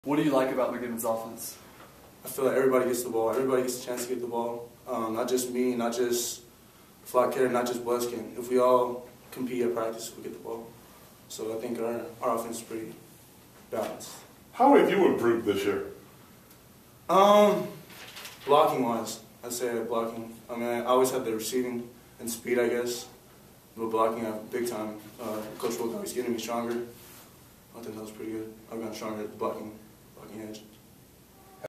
Salanoa - Sept. 25
Tiai Salanoa discusses what its like playing offense for Coach McGiven and how he's improved as a player.